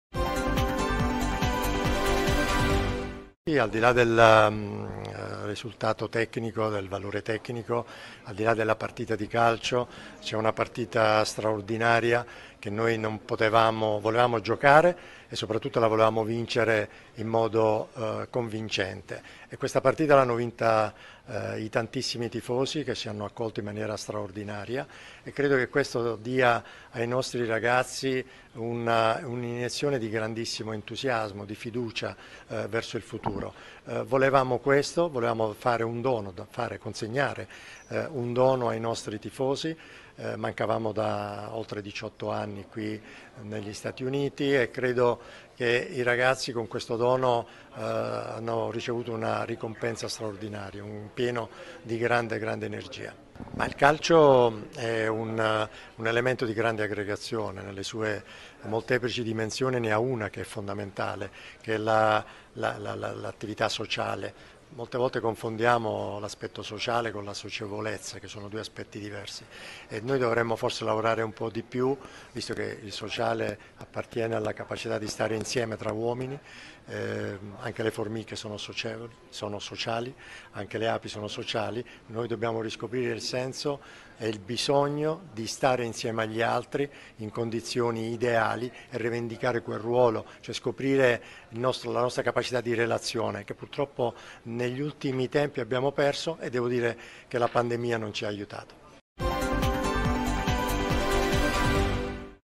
Gravina "Col Venezuela hanno vinto i tifosi"
NEW YORK (STATI UNITI) (ITALPRESS) - "I tifosi  ci hanno accolto in maniera straordinaria. Credo che questo dia ai nostri ragazzi una grande iniezione di fiducia per il futuro". Sono le parole del presidente della Figc, Gabriele Gravina, in visita al Palazzo di Vetro dell'Onu a New York in occasione della trasferta azzurra negli States.

xo9/glb/red (video di Stefano Vaccara)